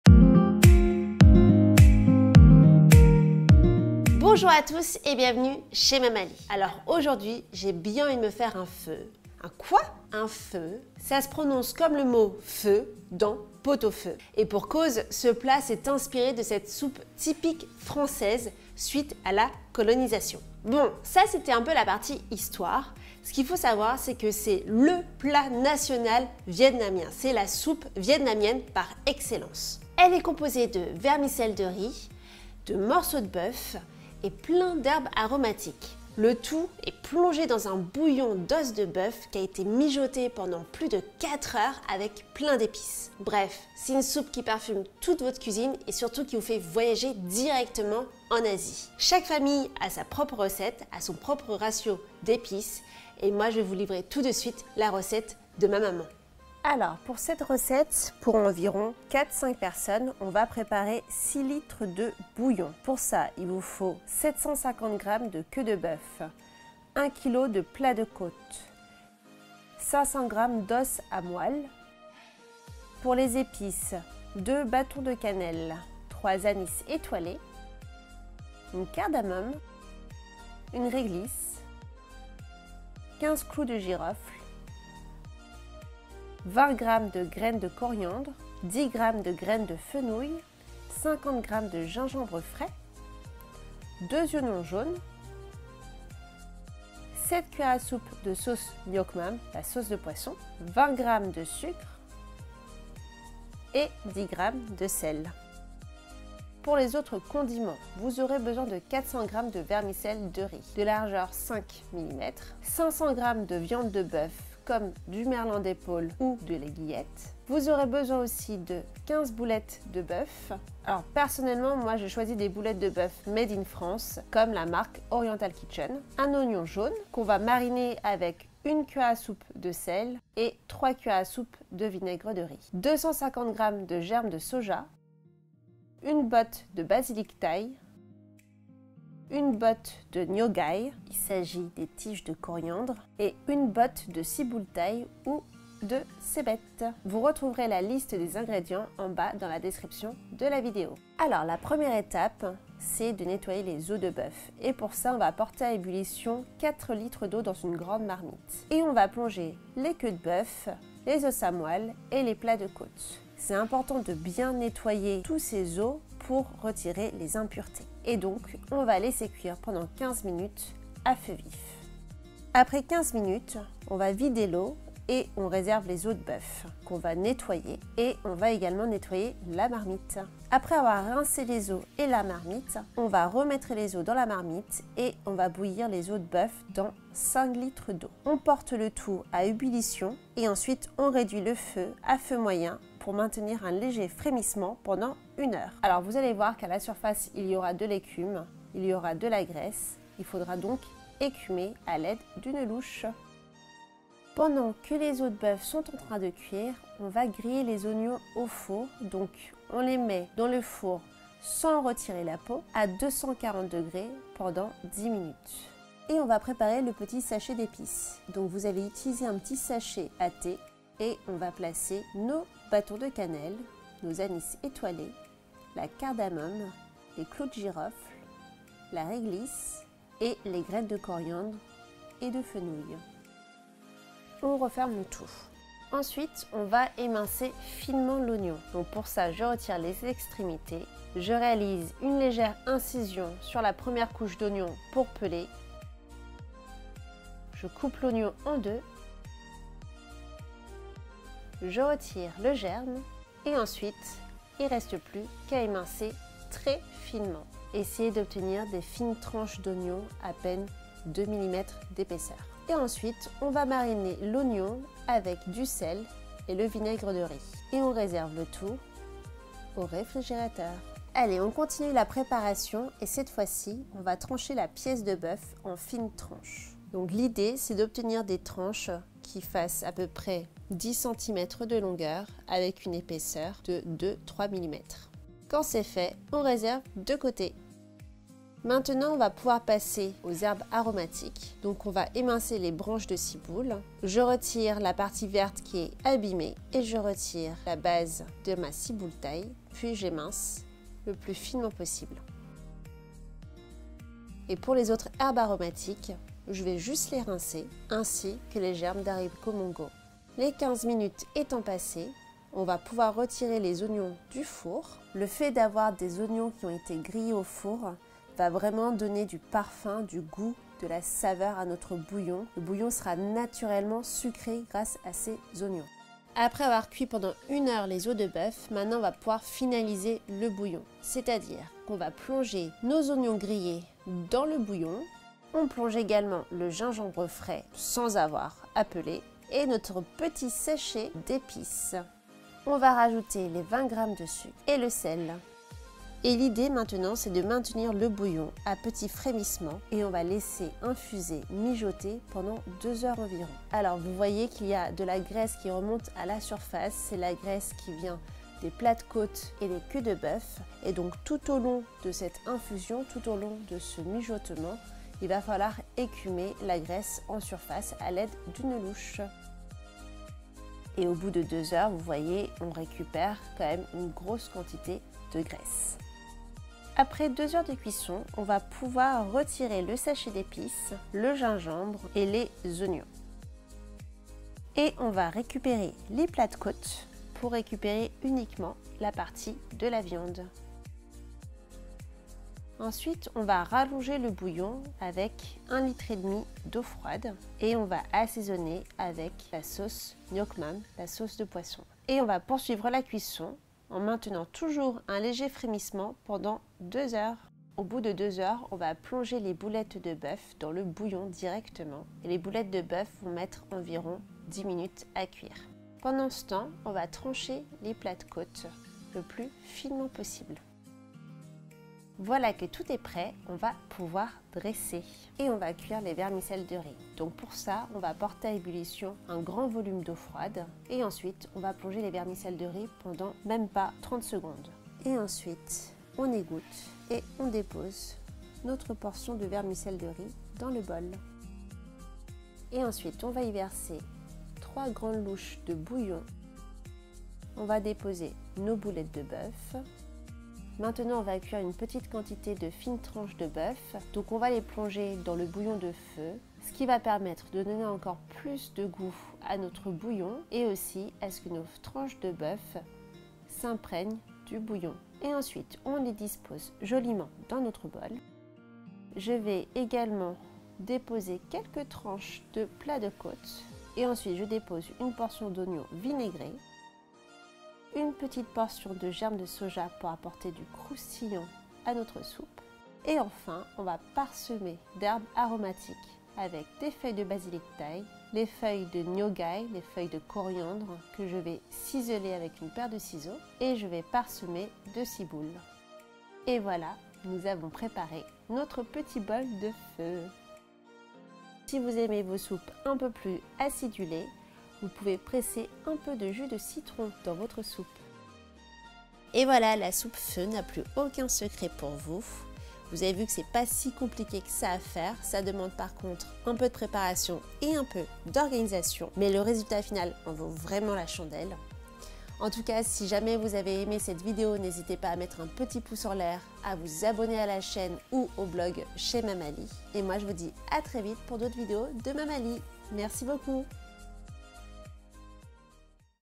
SOUPE PHÔ - RECETTE TRADITIONNELLE
Direction le Vietnam avec la soupe emblématique : Phô. Il s'agit d'une soupe de vermicelles de riz avec un bouillon de boeuf qui a été mijoté pendant 4h avec plein d'épices. Dans cette vidéo, je vous montre comment réaliser le bouillon de boeuf dans les règles de l'art.

Voici la liste des ingrédients :
Pour le bouillon (6L de bouillon Phô / 4 personnes)
- 750g de queue de boeuf
- 1kg de plat de côte
- 500g d'os à moelle
- 2 bâtons de cannelle
- 1 cardamome noire
-  3 anis étoilées
- 15 clous de girofle
- 1 réglisse
- 20g de graines de coriandre
- 10g de graines de fenouil
- 2 oignons
- 50g de gingembre frais
- 7 c. à soupe de Sauce Nuoc Mam (sauce de poisson)

Condiments :
- 400g de vermicelles de riz
- 500g de viande de boeuf
- 15 boulettes de Boeuf (Oriental Kitchen)
- 250g de germes d'haricot mungo
- 1 oignon jaune
- 3 c. à soupe de vinaigre de riz
- 1 c. à soupe de sel
- Herbes Aromatiques : Ciboule Thaï, Basilic Thaï et Tiges de Coriandre Ngo Gai

Chapitres :
00:00 Intro
00:57 Liste des ingrédients
02:31 Nettoyer les os de boeuf
03:02 Préparation du bouillon
03:28 Faire suer les oignons
03:37 Le sachet d'épices pour phô
03:57 Préparation des condiments
07:05 Cuisson des vermicelles de riz
07:11Dressage

SOUPE PHÔ  - RECETTE VIETNAMIENNE TRADITIONNELLE